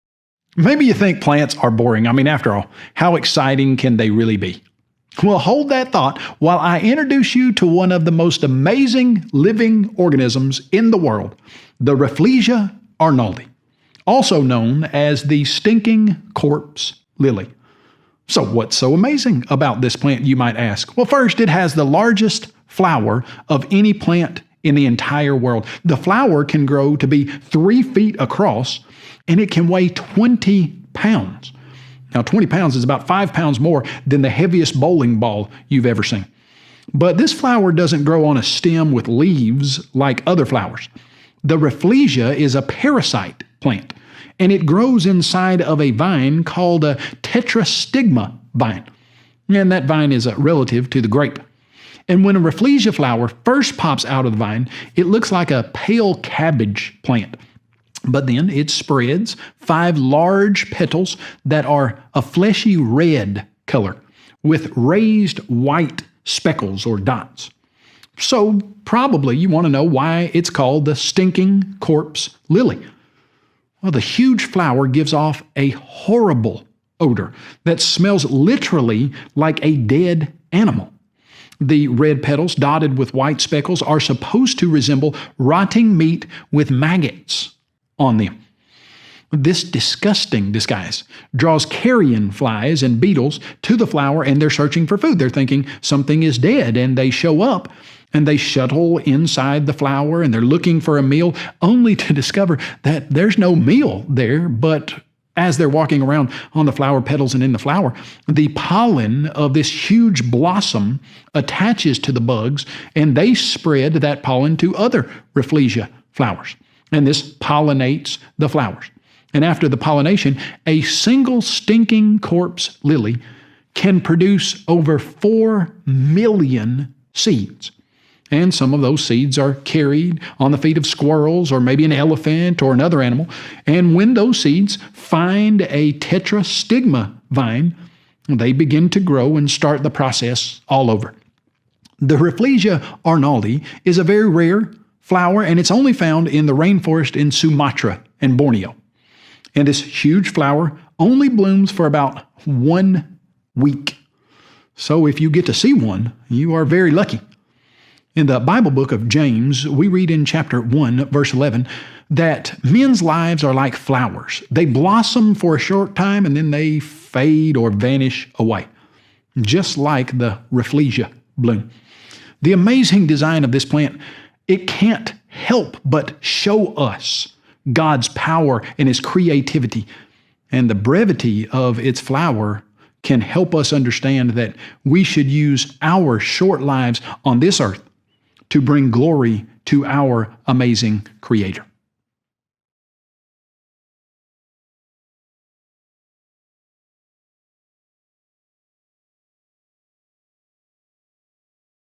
Hoa A Nặc Nhĩ - Hoa Xác Thối
Bạn có thể nghĩ rằng thực vật thật nhàm chán, bởi vì thực vật có thể thú vị đến mức nào? Tuy nhiên, hãy tham gia cùng Kyle Butt khi anh ấy giới thiệu bạn với Hoa A Nặc Nhĩ - Hoa Xác Thối. Loại cây ký sinh khổng lồ, có mùi kỳ lạ, kỳ lạ này sẽ khiến bạn ngạc nhiên vì sự sáng tạo của Chúa thực sự tuyệt vời như thế nào!